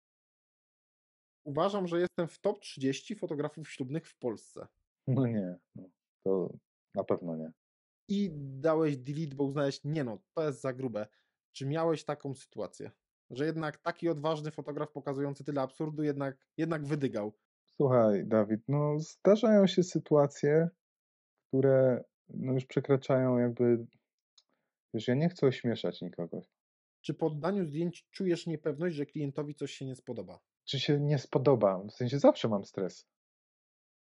Marcin Urbanowicz fotograf ślubny unikający schematów - fragment rozmowy.
Rozmowa z Marciniem Urbanowiczem, którego fotografia ślubna wyrywa się poza schematy. Spontaniczność i poczucie humoru to jego niewątpliwe zalety. Jego reportaż ślubny to kadry przede wszystkim autentyczne zdjęcia z autorską wizją.